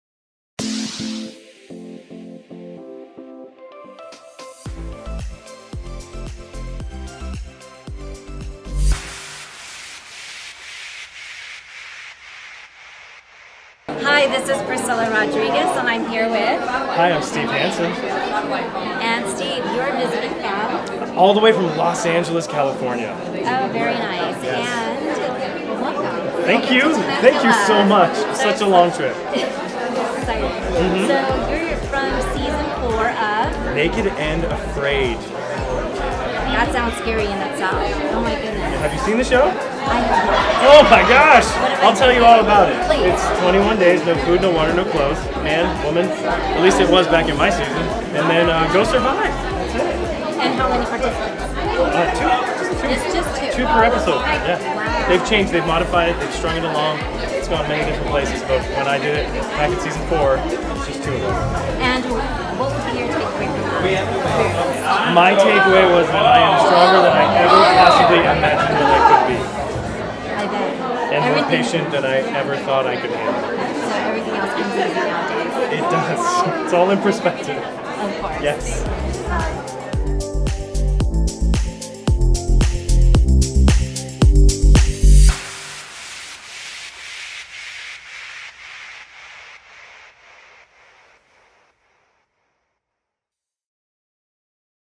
Reality Rally 2022 with Priscilla Rodriguez and Steve hansen
Reality Rally 2022 with our Finest Women in Real Estate Priscilla Rodriguez interviewing one of the National TV Reality Stars Steve Hansen!